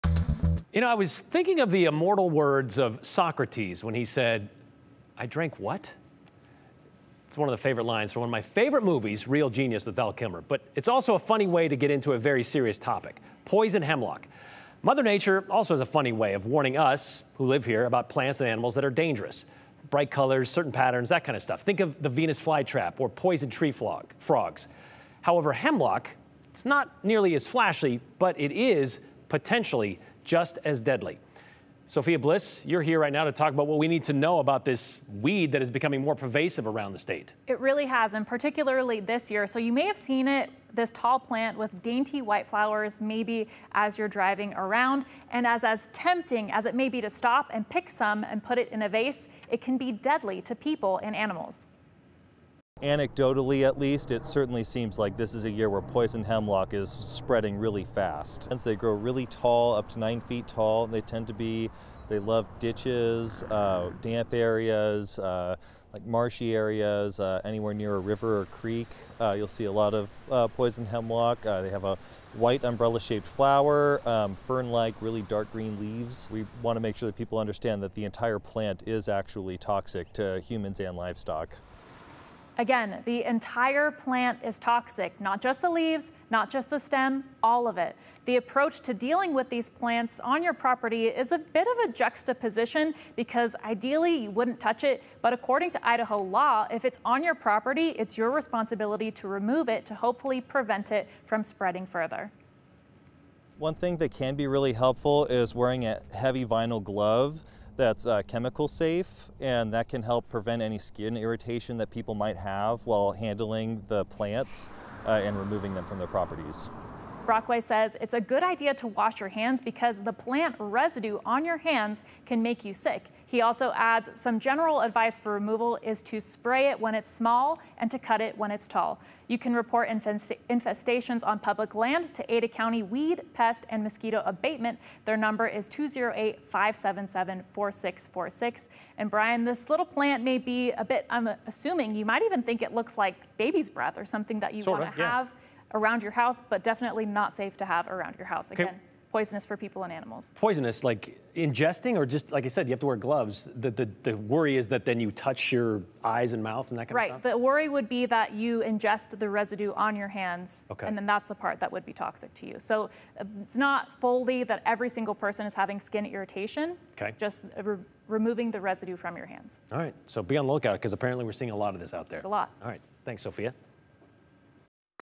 Appearances may be deceiving: Poison hemlock emerges around Ada County
An unassuming weed that can be deadly to people and animals may be in your backyard or pasture.